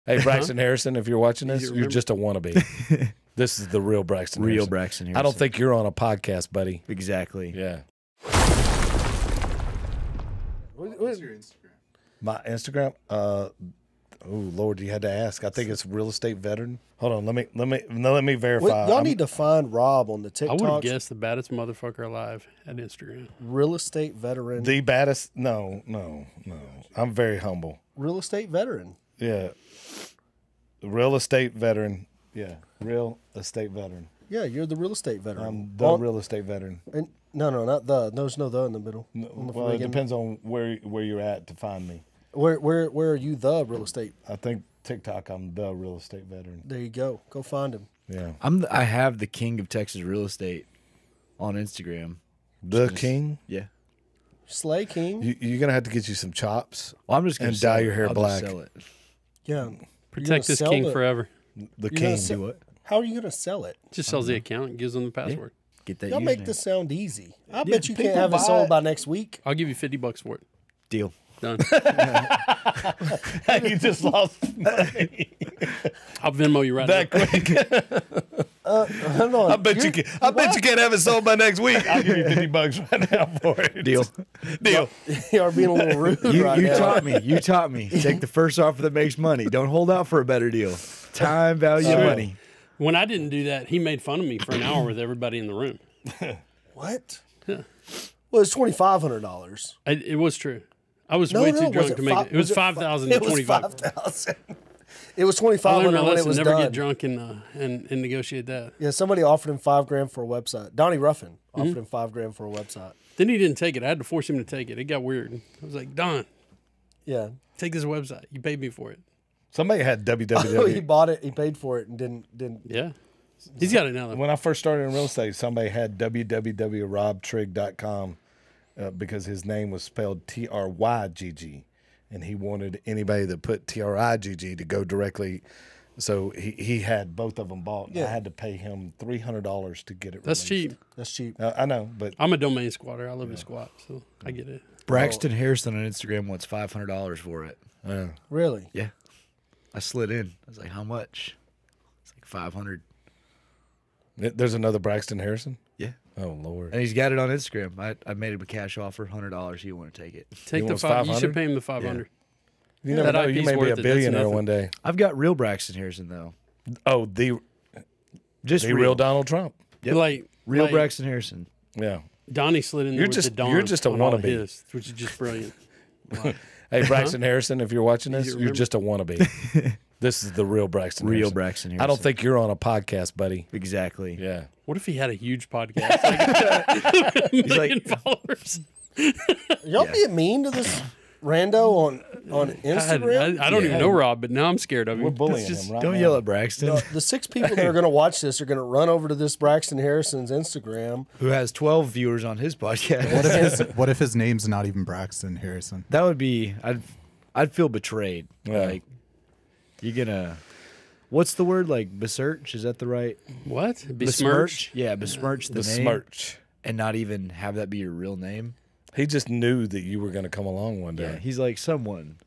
When Your Name is Already Taken on Instagram
In this clip, we discuss social media usernames and the struggles of trying to get the one you want. Braxton and Rob mention their experiences with buying usernames and URLs from other people, and people using handles that aren't even their real names.